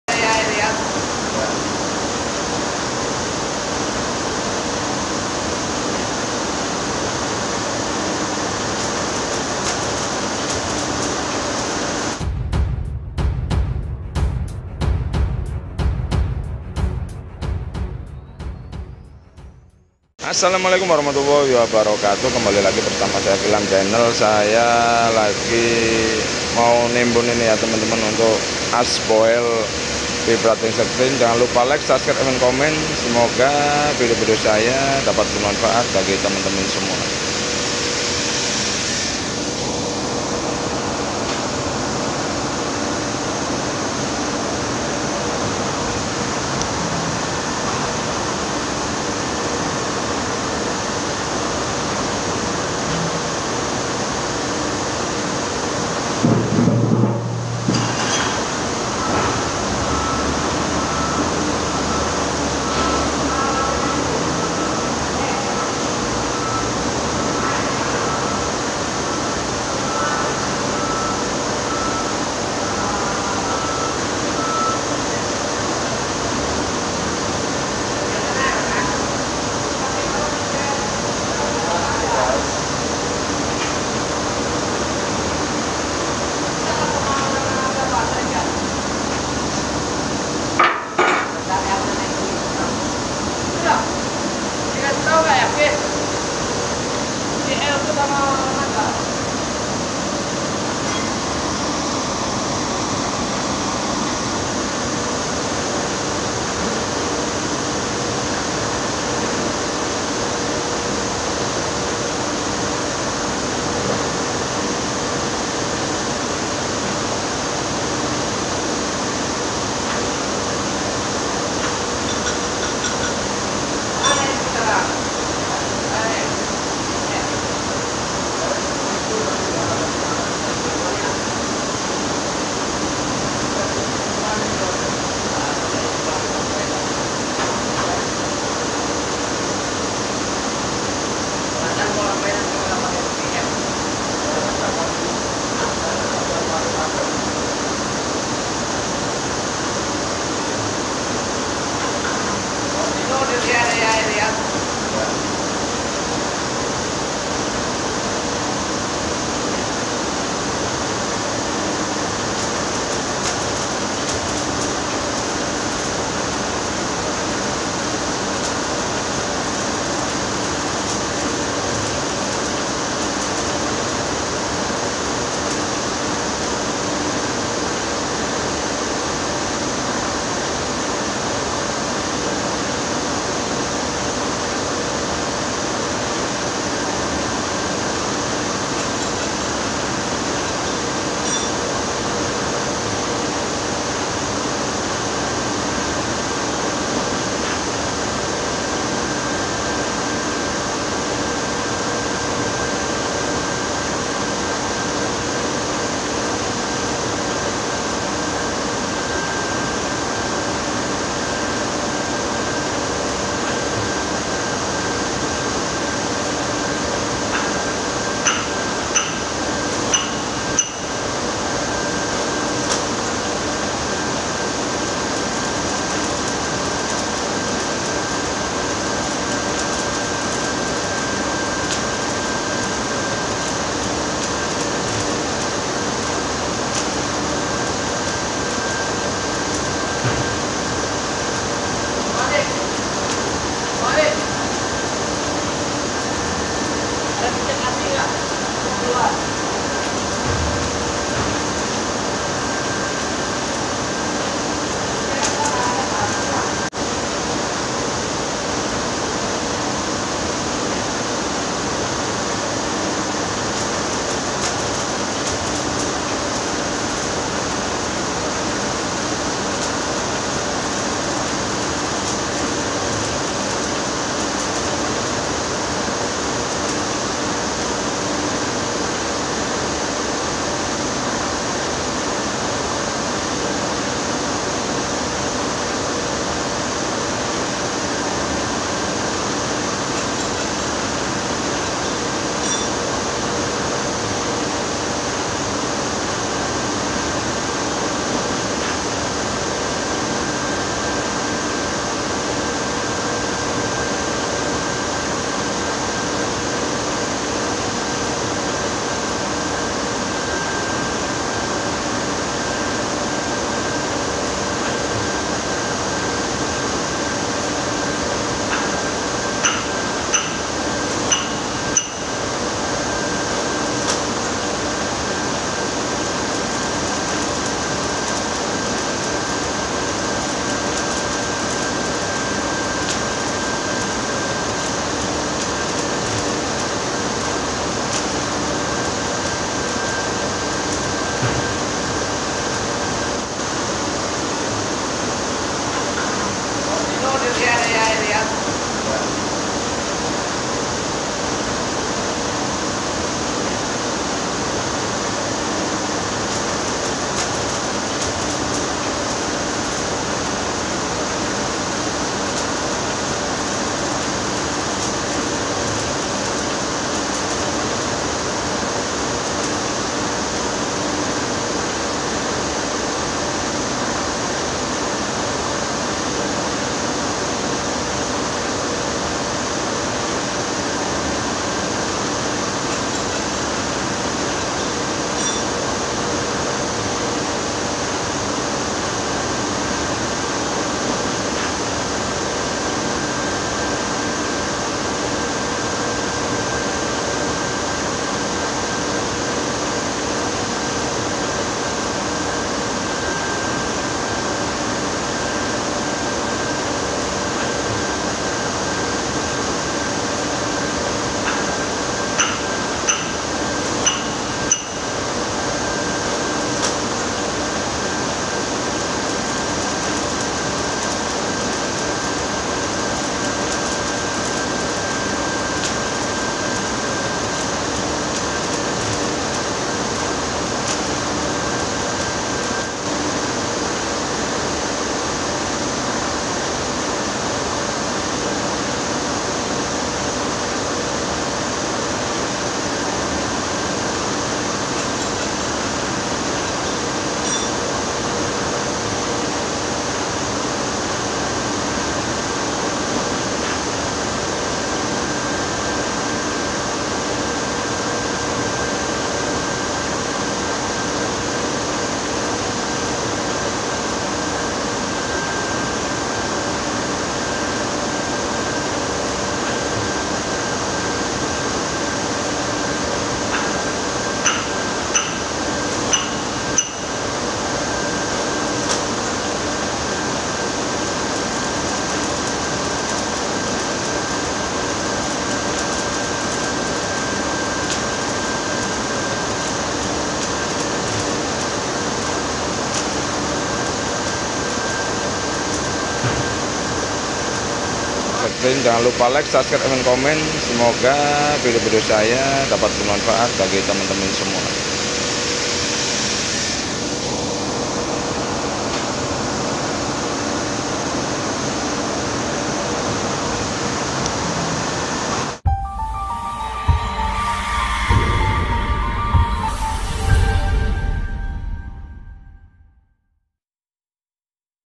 PENGELASAN AS BOHEL VIBRATING YANG RUSAK
Assalamualaikum warohmatullahi wabarokatu berjumpa bersama saya KILAM CHANNEL dalam video kali ini saya pengelasan bohel vibrating selamat menonton 🙏🙏🙏